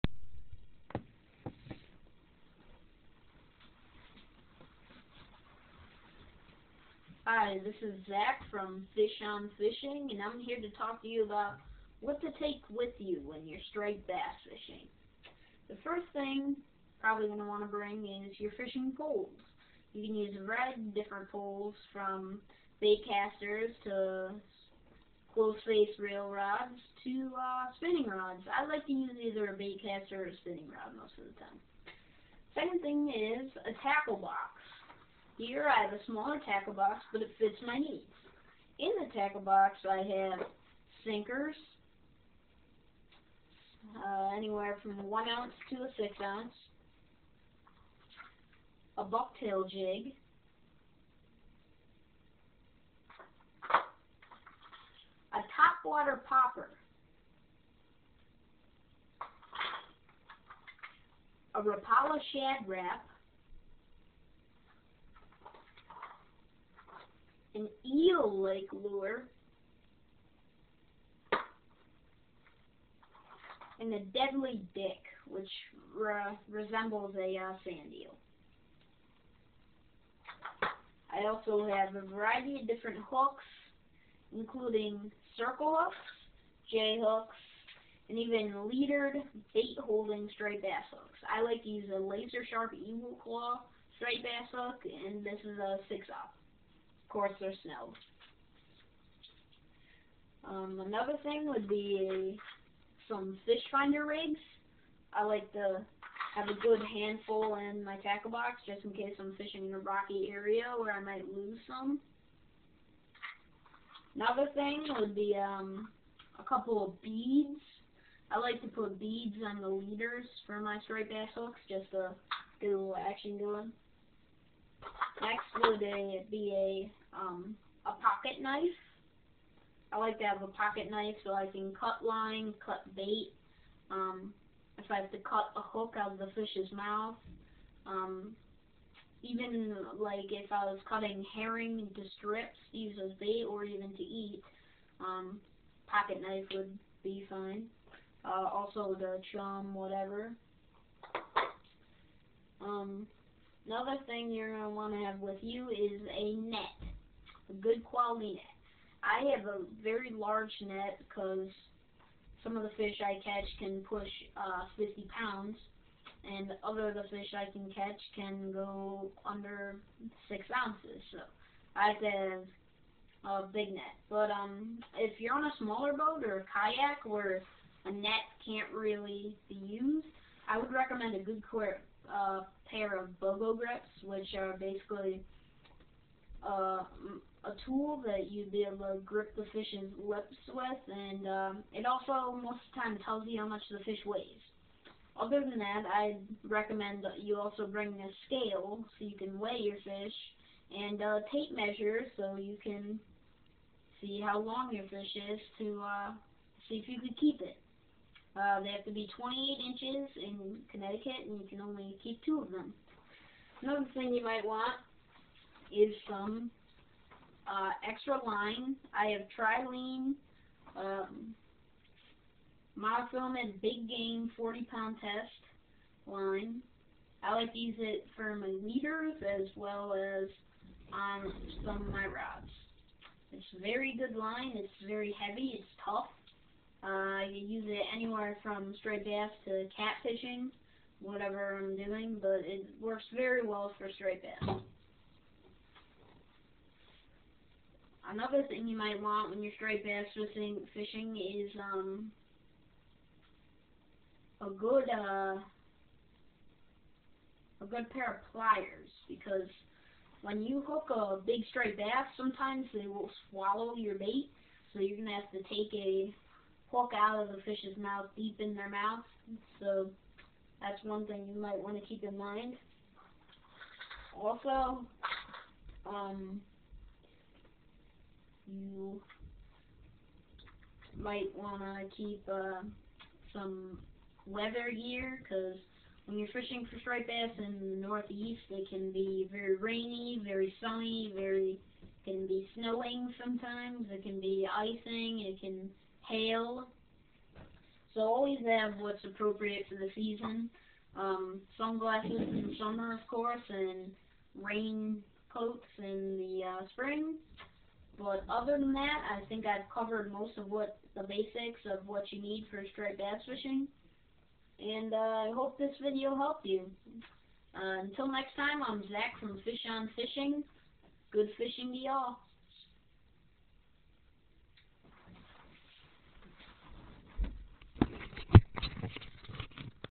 FishOnFishing: What To Bring Striped Bass Fishing
I was bored so i made a video about what to bring when you go striped bass fishing, i hoped this helped all you begginer anglers.